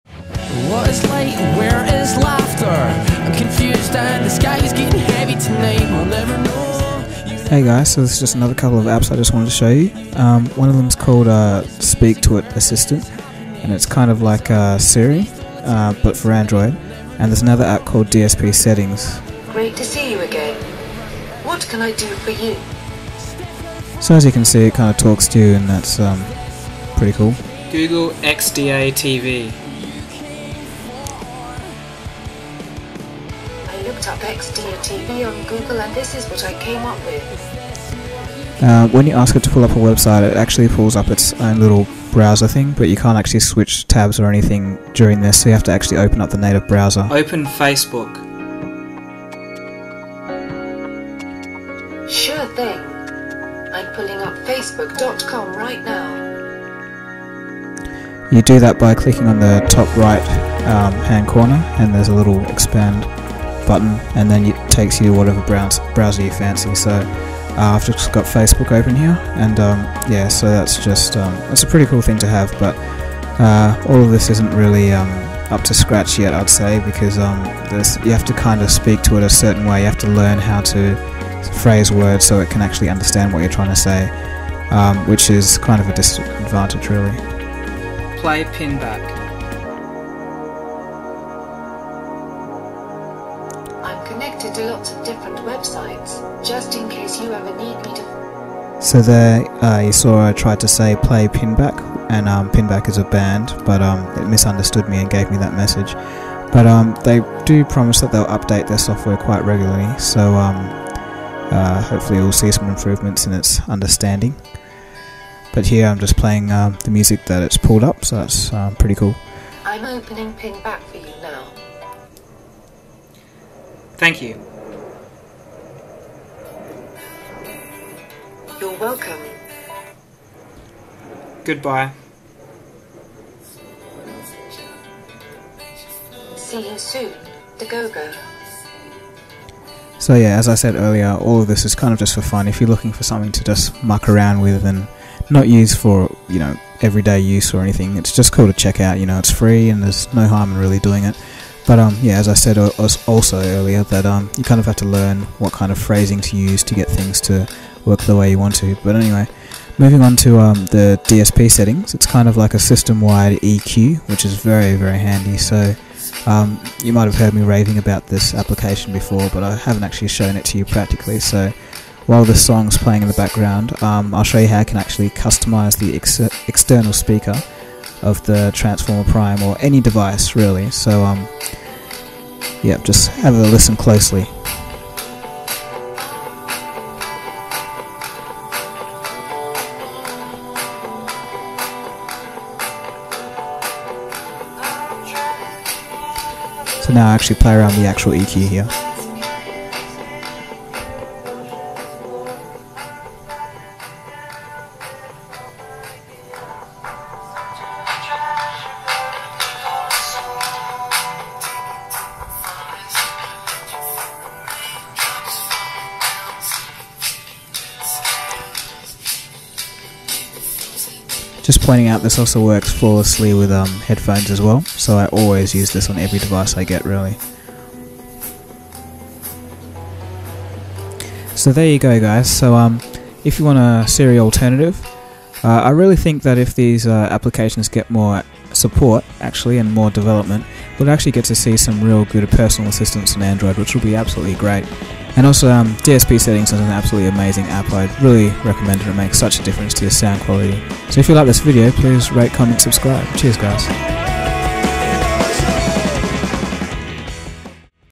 Siri Alternative On Transformer Prime (Plus Custom EQ)
Apps:

- DSP settings/ DSP manager
(Custom EQ across ALL apps, yes including music, video and youtube)

-Speaktoit assistant
(Siri alternative)